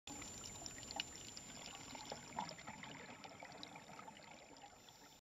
BON031 - Lac Bay - biologist & Conch
Biologist Sabine Engel investigating Conch underwater